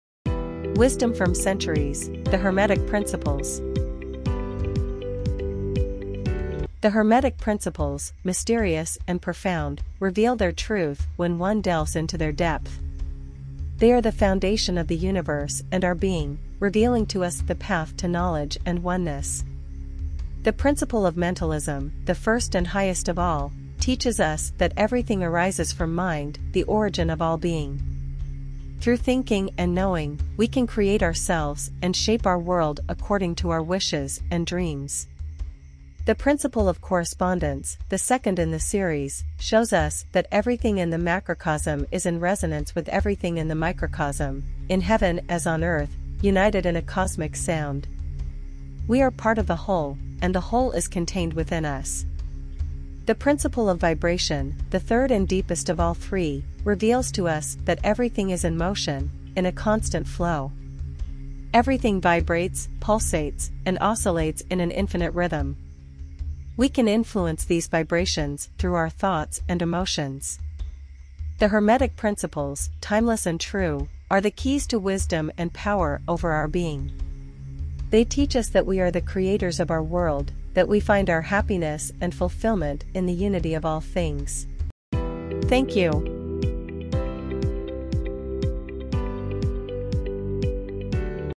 Wisdom from centuries: The Hermetic Principles
The Hermetic Principles, mysterious and profound, reveal their truth when one delves into their depth. They are the foundation of the universe and our being, revealing to us the path to knowledge and oneness.

The Principle of Mentalism, the first and highest of all, teaches us that everything arises from mind, the origin of all being. Through thinking and knowing, we can create ourselves and shape our world according to our wishes and dreams.

The Principle of Correspondence, the second in the series, shows us that everything in the macrocosm is in resonance with everything in the microcosm, in heaven as on earth, united in a cosmic sound. We are part of the whole, and the whole is contained within us.

The Principle of Vibration, the third and deepest of all three, reveals to us that everything is in motion, in a constant flow. Everything vibrates, pulsates, and oscillates in an infinite rhythm. We can influence these vibrations through our thoughts and emotions.

The Hermetic Principles, timeless and true, are the keys to wisdom and power over our being. They teach us that we are the creators of our world, that we find our happiness and fulfillment in the unity of all things.

The White Belief - A shaman is someone who mediates between worlds.

Shamanism is based on spiritual energy and works on the principle of the law of resonance. The world is imbued with endless spirits, and these spiritual energies inhabit everything, from trees and stones to rivers and mountains. Everything is imbued with spiritual energy that generates its own vibrations.

Also, everything we carry in ourselves, our experiences, memories, and feelings, can be traced back to spiritual energy that resonates with its own unique frequency. Your genetic memory also holds the spiritual energies of your ancestors and forebears. You can observe this when you look at symbolic images and feel their meaning intuitively. This is the moment when you don't understand with your mind but comprehend with your heart. This symbolic power is deeply embedded in all of us, in the collective unconscious, because long before there was today's language, there was the language of sacred symbols.

When you have a question or need help, the world of symbols is ready to give you the help you need. These symbols not only symbolize reality but can also initiate a transformation process that changes reality. This power and wisdom allow us to understand the present, heal the past, and influence the future.

Start creating your own life!